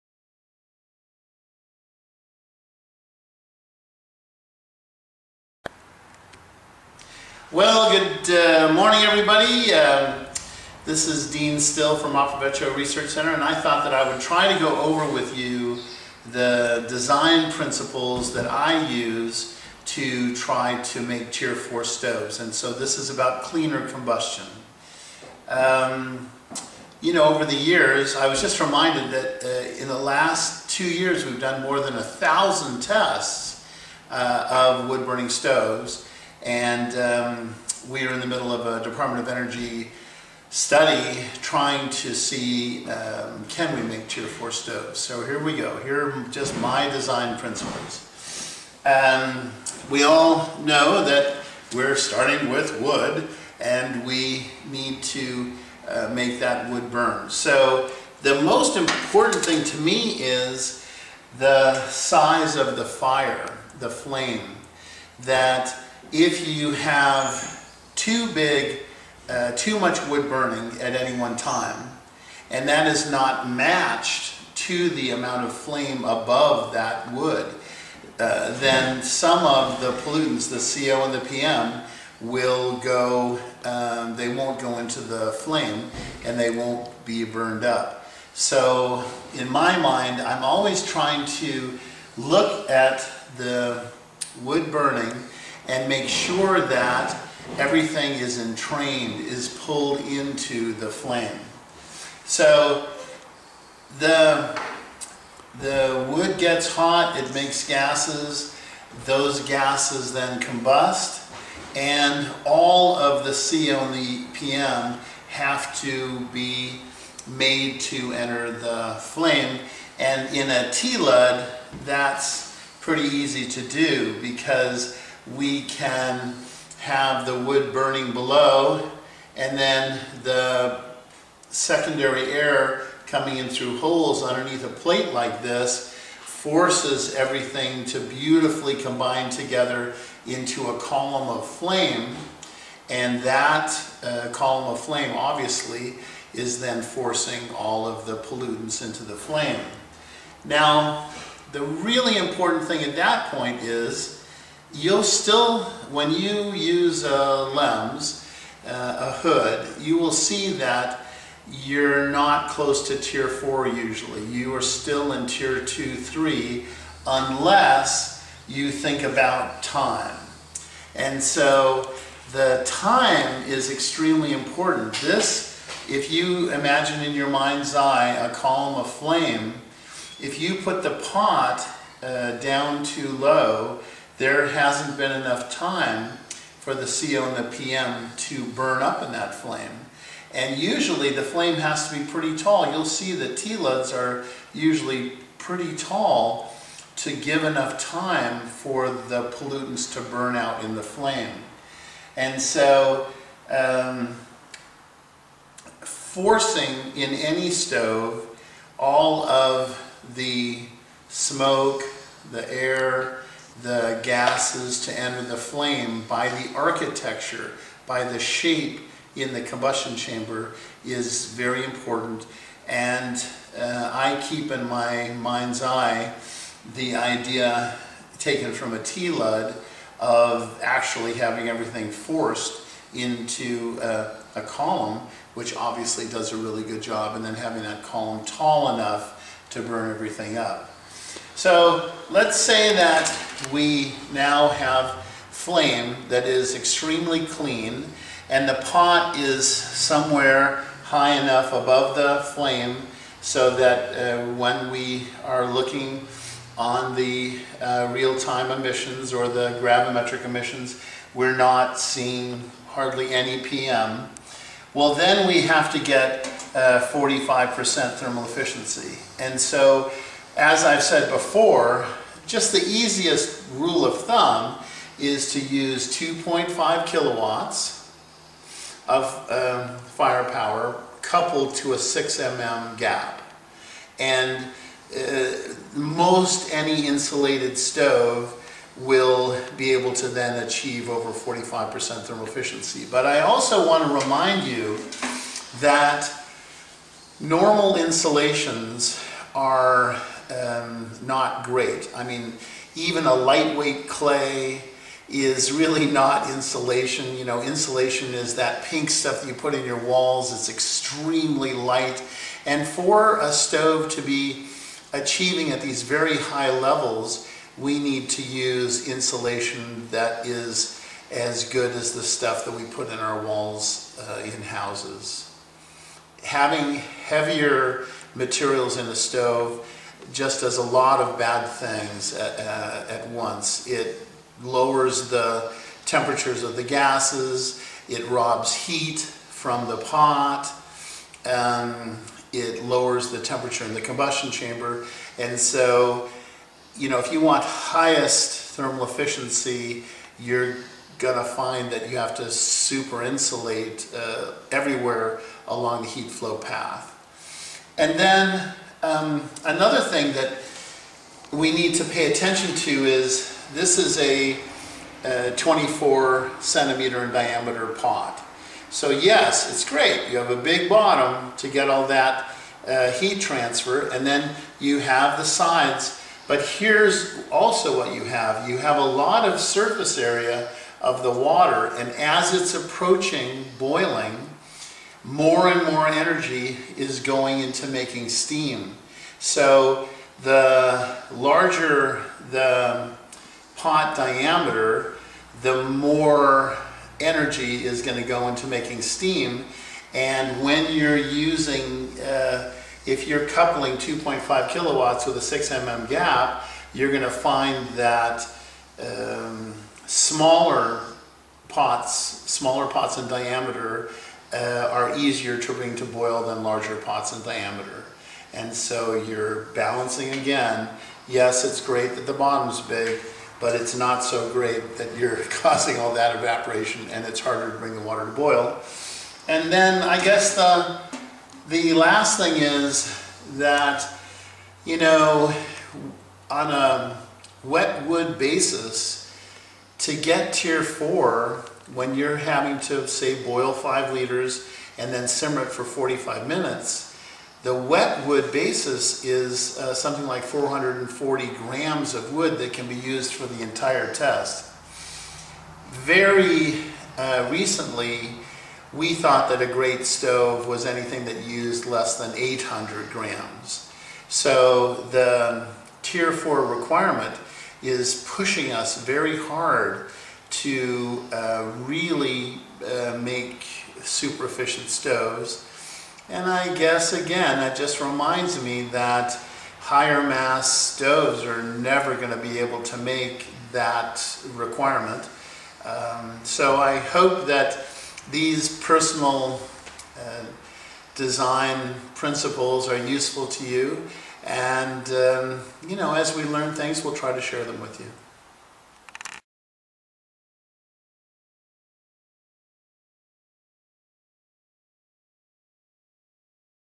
Cleaner Combustion Principles with Dean Still
In the latest installment in ARC's video series Dean Still discusses clean combustion principles and personal stove design ideology.

If you are interested in more information on clean cookstove technology or emissions testing, please visit Aprovecho's official